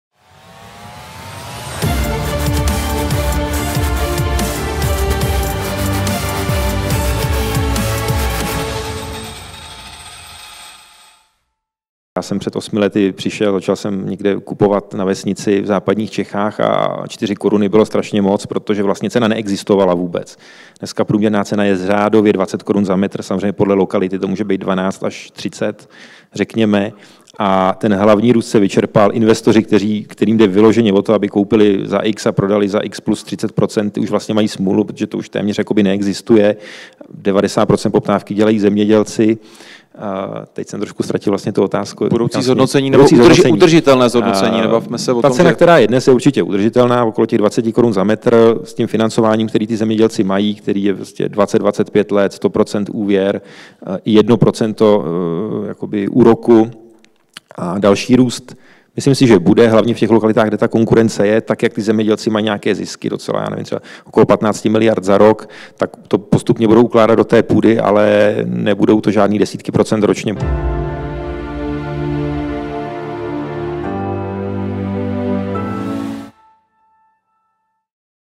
Jaklín (ČPF): Dynamické zdražování české zemědělské půdy je minulostí
Růst cen o desítky procent za rok je v případě české zemědělské půdy již minulostí. Většinu poptávky vytvářejí sami zemědělci, uvedl na Investičním fóru v Praze Jiří Jaklín z Českého půdního fondu.